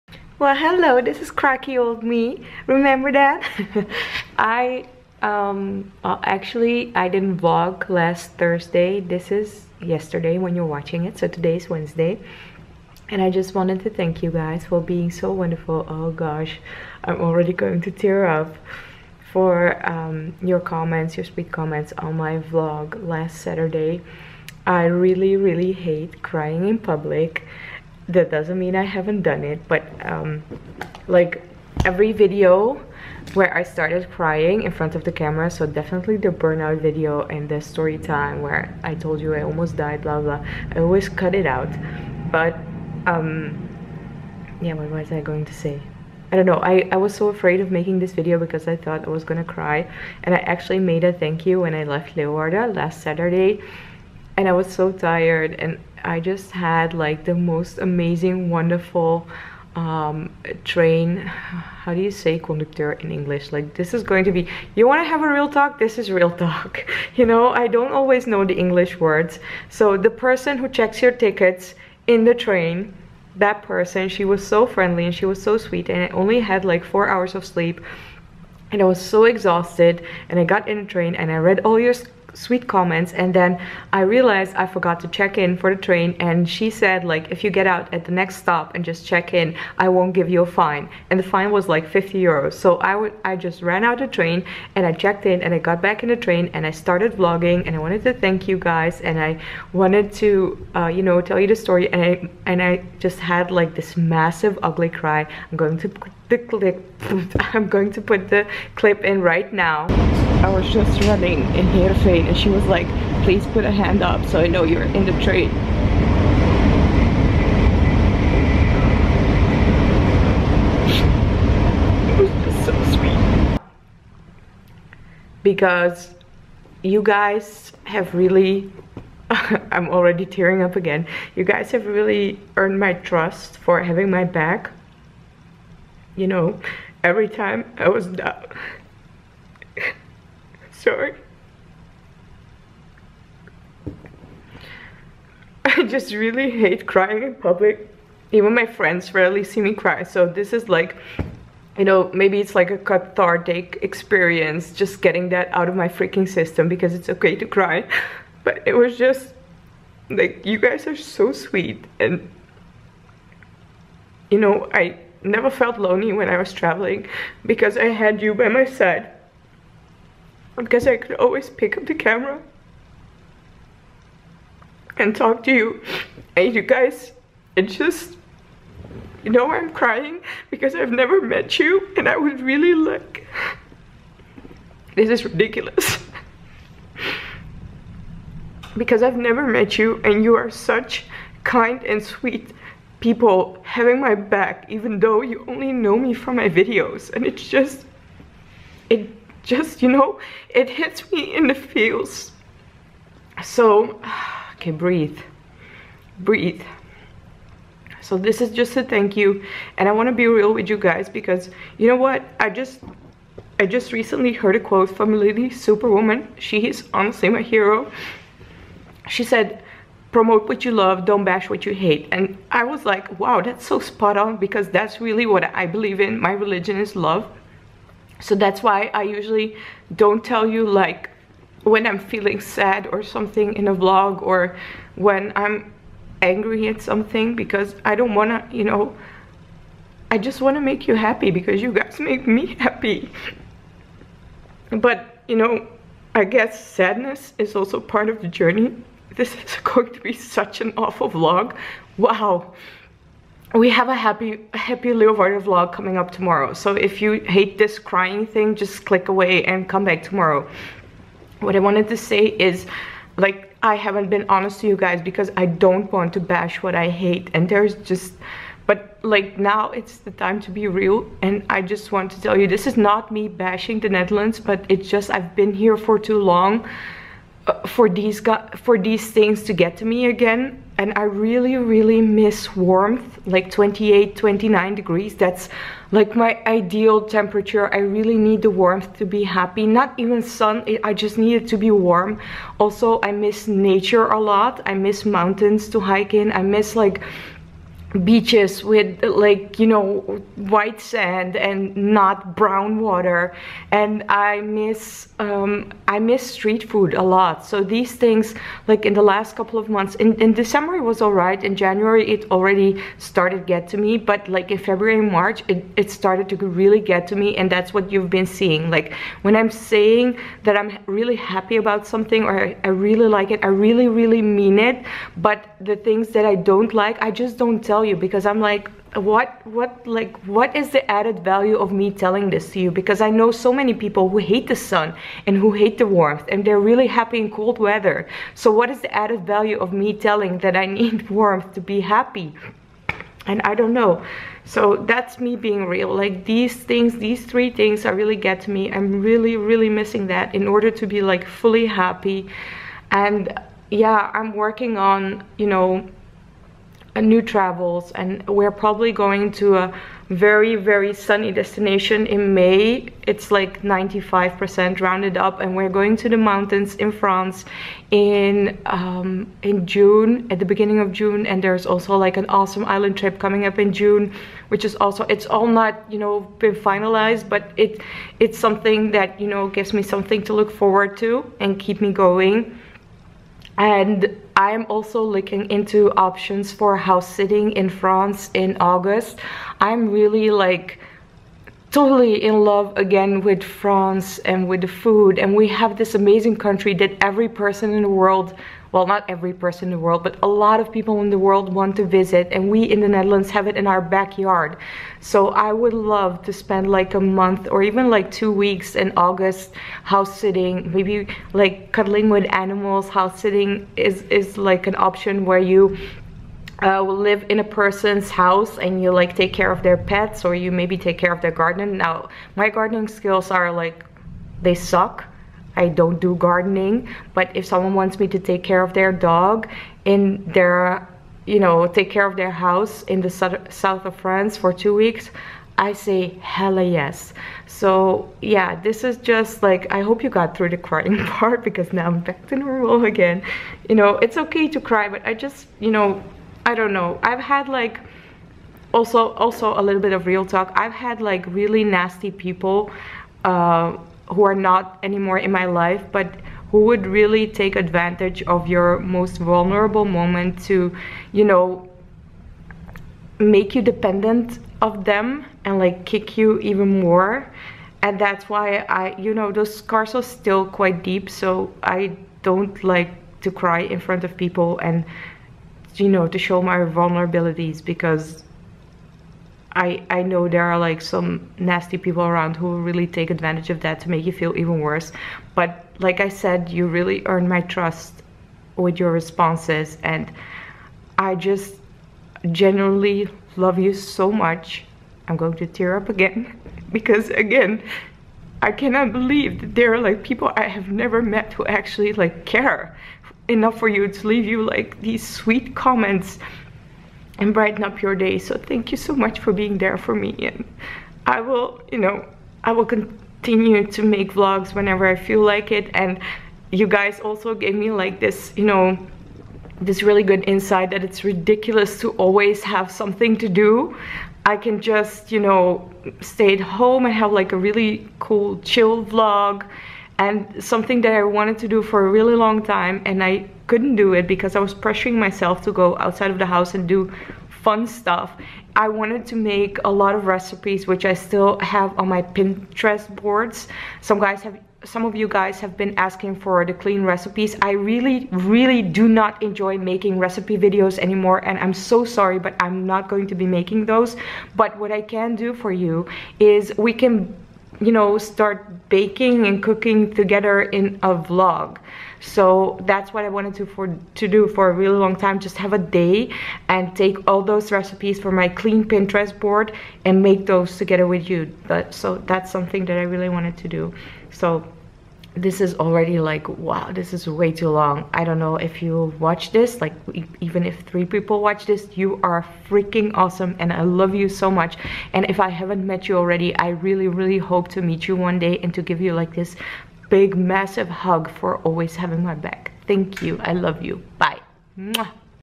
JUST A REALLY, BUT REALLY, LONG EMOTIONAL THANK YOU + REAL TALK
I just wanted to realtalk to you about why I'm not happy and not sharing this more often. Also I want to thank you for the incredibly sweet comments on my not happy vlog. I got so emotional because I can't believe people who I have never met care enough about me to leave me such supportive comments. Please bear with me through alle the rambling. I hate talking about things I don't like and I ABSOLUTELY hate crying in public. But like I said you have earned my trust to see my vulnerabilities. Again a massive thank you and big hug for being there for me. Love you!!!